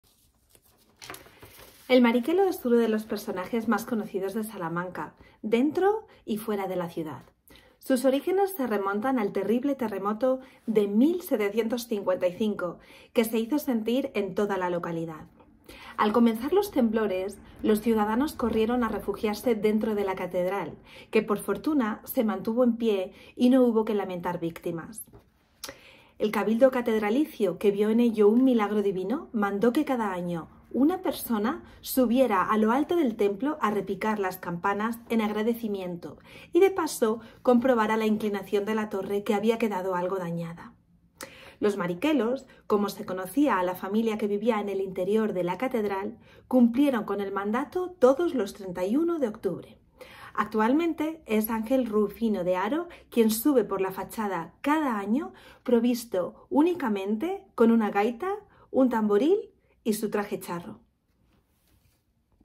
El Mariquelo
IV Lectura Colectiva en el Día Internacional del Libro. Obra: El Mariquelo.